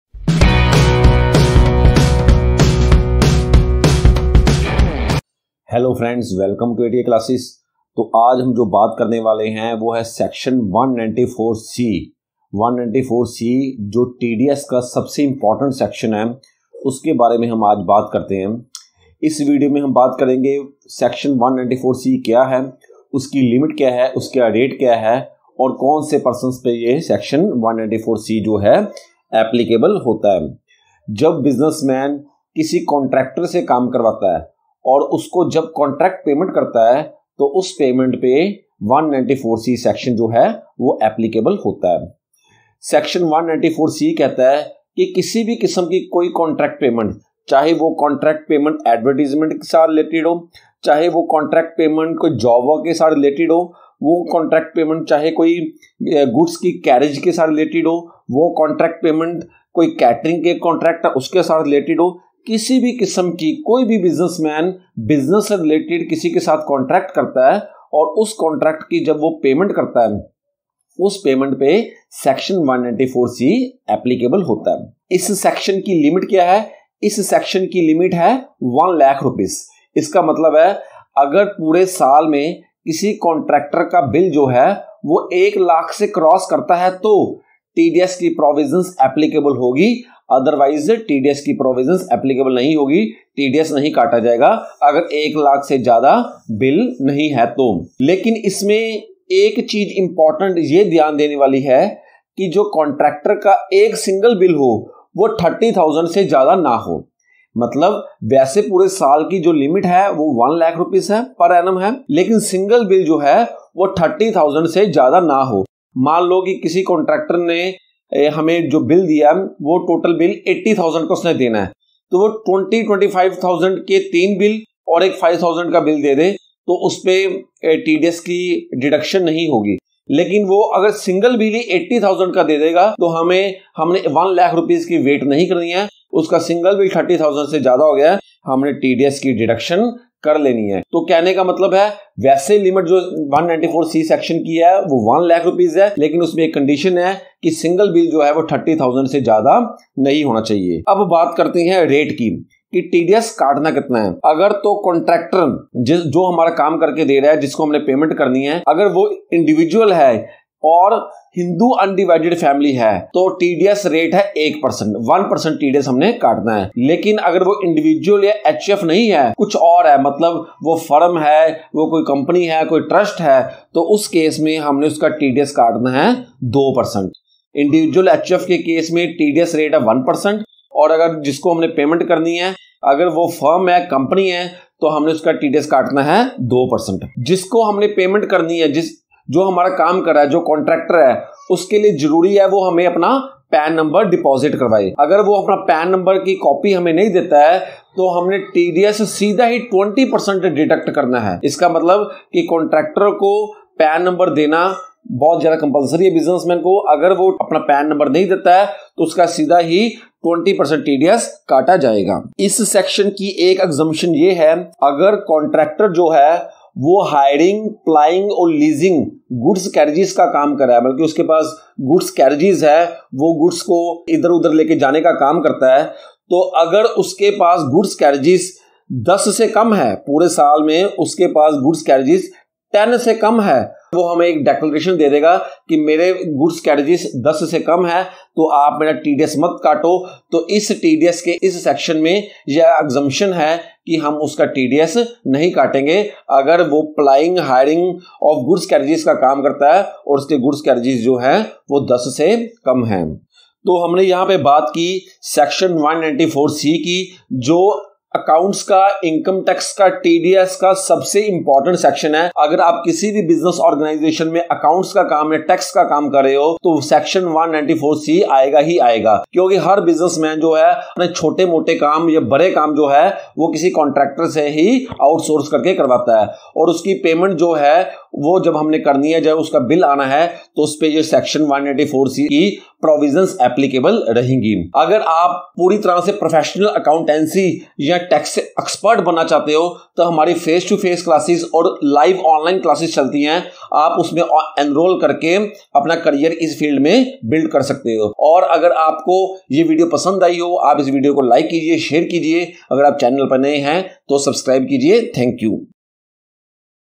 Section 194C of TDS | TDS ON CONTRACT PAYMENTS
SECTION 194C

 1. TDS is deducted on Contract Payments in the Business.
 2. Limit for Contract Payments is Rs. 1,00,000 p.a
But there is one condition is that 1 Bill should not exceed Rs. 30,000.

 3. Rate of TDS for Individuals/HUF is 1%
Rate of TDS for Firms/Companies is 2%.

 4. If contractor does not furnish his Pan No, then 20 % TDS is deducted.

 5. If contractor engaged in business of hiring, plying and leasing goods carriage and does not own more than 10 goods carriages during the year.